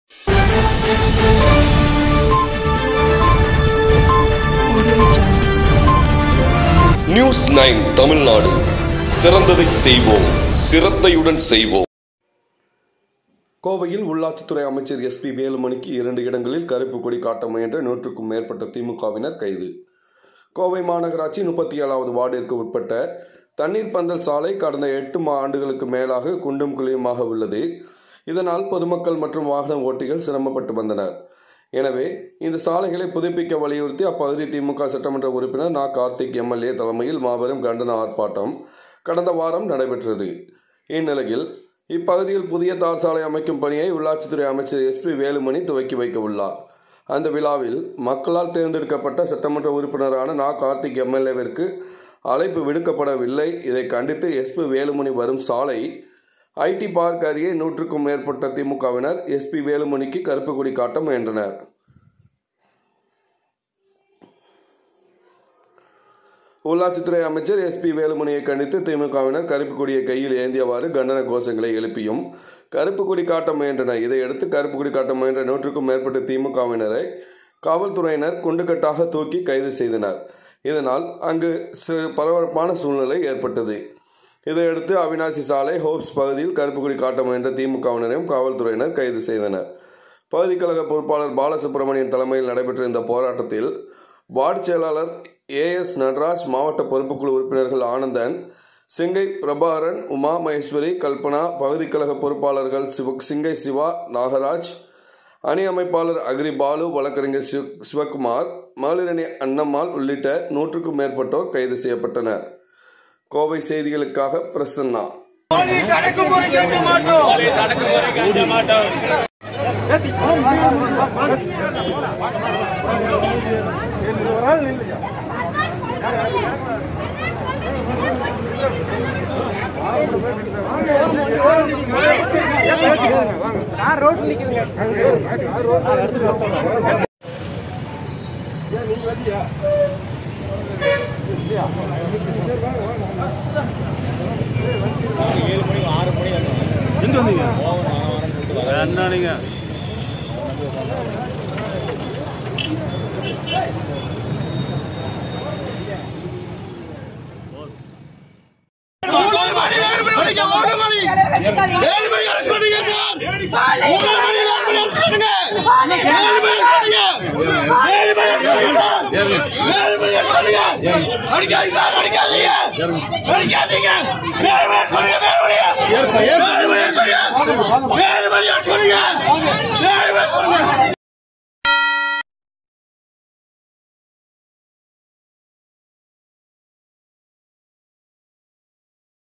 உள்ளாட்சித்துறை அமைச்சர் எஸ்பி வேலுமணிக்கு கருப்பு கொடி காட்ட முயன்ற திமுகவினர் கைது
கோவை மாநகராட்சி, 37 வது வார்டிற்கு உட்பட்ட தண்ணீர்பந்தல் சாலை கடந்த 8 ஆண்டுகளுக்கு மேலாக குண்டும் குழியுமாக உள்ளது. இதனால், பொதுமக்கள், மற்றும் வாகன ஓட்டிகள் சிரம்மப்பட்டு வந்தனர். எனவே, இந்த சாலைகளை புதுப்பிக்க வலியுறுத்தி அப்பகுதி திமுக சட்டமன்ற உறுப்பினர் நா.கார்த்திக் எம்எல்ஏ தலைமையில் மாபெரும் கண்டன ஆர்ப்பாட்டம் கடந்த வாரம் நடைபெற்றது. இந்நிலையில், இப்பகுதியில், புதிய தார்சாலை அமைக்கும் பணியை உள்ளாட்சித்துறை அமைச்சர் எஸ்பி.வேலுமணி துவக்கி வைக்க உள்ளார். அந்த விழாவில், மக்களால் தேர்ந்தெடுக்கப்பட்ட சட்டமன்ற உறுப்பினரான நா.கார்த்திக் எம்எல்ஏ விற்கு அழைப்பு விடுக்கப்பட வில்லை. இதை கண்டித்து எஸ்பி.வேலுமணி வரும் சாலை ஐடி பார்க் அருகே 100 க்கும் மேற்பட்ட திமுகவினர், எஸ்பி.வேலுமணிக்கு கருப்பு கொடி காட்ட முயன்றனர். உள்ளாட்சித்துறை அமைச்சர் எஸ்பி.வேலுமணியை கண்டித்து, திமுகவினர் கருப்பு கொடியை கையில் ஏந்தியவாறு, கண்டன கோஷங்களை எழுப்பியும் கருப்பு கொடி காட்ட முயன்றனர். இதையடுத்து, கருப்பு கொடி காட்ட முயன்ற 100 க்கும் மேற்பட்ட திமுகவினரை காவல்துறையினர் குண்டு கட்டாக தூக்கி கைது செய்தனர். இதனால், அங்கு பரபரப்பான சூழ்நிலை ஏற்பட்டது.

இதையடுத்து, அவினாசி சாலை, ஹோப்ஸ் பகுதியிலும் கருப்பு கொடி காட்ட முயன்ற திமுகவினரையும் காவல்துறையினர் கைது செய்தனர்.

பகுதி கழக பொறுப்பாளர் பாலசுப்பிரமணியம் தலைமையில் நடைபெற்ற இந்த போராட்டத்தில், வார்டு செயலாளர் ஏஎஸ்.நடராஜ் , மாவட்ட பொறுப்புக்குழு உறுப்பினர்கள் ஆனந்தன், சிங்கை பிரபாகரன், உமா மகேஸ்வரி, கல்பனா, பகுதி கழக பொறுப்பாளர்கள் சிங்கை சிவா,  நாகராஜ், அணி அமைப்பாளர் அக்ரி பாலு, வழக்கறிஞர் சிவக்குமார், மகளிரணி அன்னம்மாள் உள்ளிட்ட 100 க்கும் மேற்பட்டோர் கைது செய்யப்பட்டனர்.